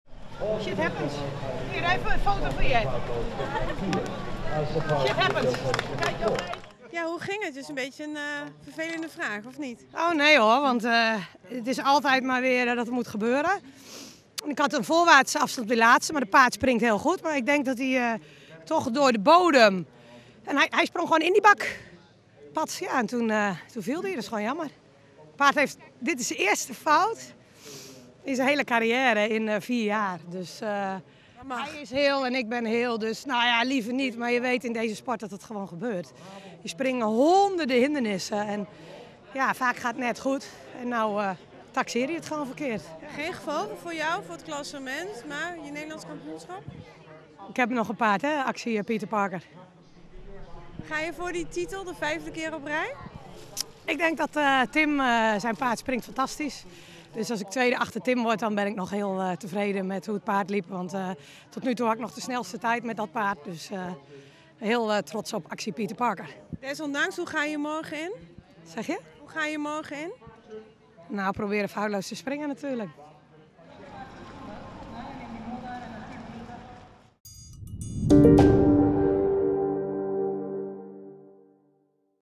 Tim Lips en Alice Naber-Lozeman na cross-country in top vier bij Military Boekelo
Nederlands succes op de derde dag van Military Boekelo. Tim Lips en Alice Naber-Lozeman staan na het tweede onderdeel, de cross-country, op de derde envierde plek in het klassement. Tim Price uit Nieuw-Zeeland nam de leiding over van de Chinees Alex Hua Tian.